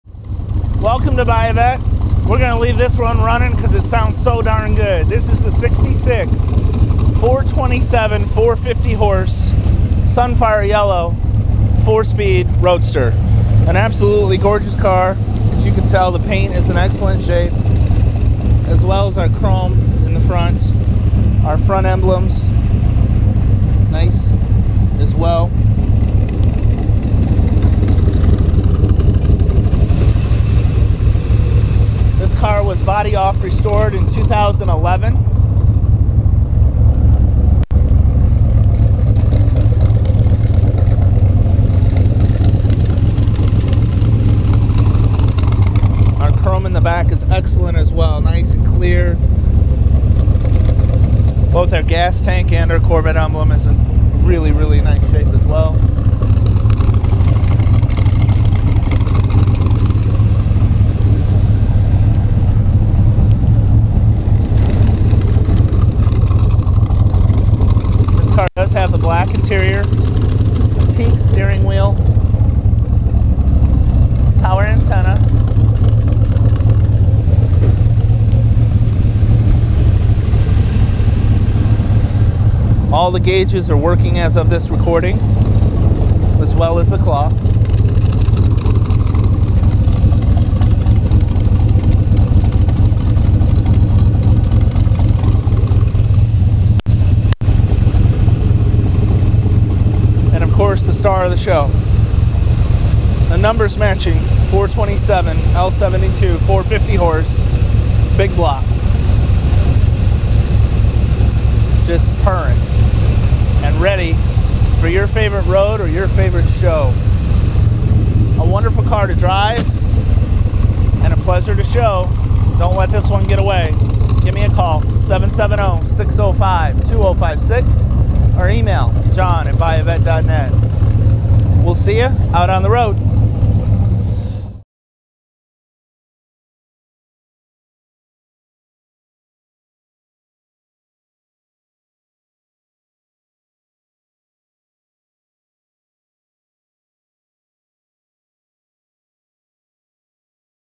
1966 Corvette 427/450hp Convertible
1966 Corvette 427/450hp Convertible For Sale

Code correct Sunfire Yellow exterior, code correct Black vinyl interior, Black convertible top. Numbers matching 450hp 427ci L72 engine, 4 speed manual transmission. Features include power steering, knockoff style wheels, side exhaust, teakwood steering wheel, AM/FM radio, power antenna. Originally assembled on September 16, 1965, this 1966 Corvette 427/450hp Convertible is one of the first '66s built, and made it out the door with a 450hp rating before GM decided to reduce the rating to 425hp, presumably for insurance purposes. This car received a very extensive BODY-OFF restoration by a very knowledgeable and NCRS-trained gentleman, with great attention to detail and correctness. This restoration was completed in 2011, and power steering was added to make the car easier to drive. This Protect-O-Plate documented car shows vg paint, with an absolutely stunning shine. Convertible top is good-vg, with a vg window. Chrome bumpers are vg-exc, and exterior door handles are vg. Weatherstripping is good-vg, and knockoff style wheels and associated spinners and caps are vg-exc. Coker Classic radial goldline tires show an average of 8/32nds tread depth remaining in the front, and an average of 9/32nds remaining in the rear. The interior of this 1966 Corvette 427/450hp Convertible shows vg-exc seats, carpet, and teakwood steering wheel, and vg door panels, center console, gauges, glovebox lid, and GM seatbelts. Correct appearing AM/FM radio is in place, and the clock is working with a quartz movement at the time of this write-up. The numbers matching L72 engine in this car shows final assembly date September 6, 1965. Correct air cleaner, carburetor, intake manifold, ignition shielding, valve covers, ignition wires, radiator hoses, and hose clamps are all in place, and the frame is solid and rust-free. This car has been fully serviced, and all instrument gauges, lights, turn signals, brakes, engine, transmission and rear-end are all in good operating condition. Give us a call today before someone else puts this '66 450hp drotop in their collection!